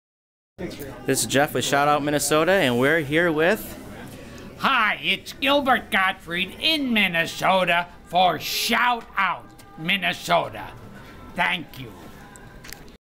GILBERT GOTTFRIED video drop with @ShoutOutMN SHOUT OUT MINNESOTA!!
Legendary comedian says hello to the fans of Minnesota.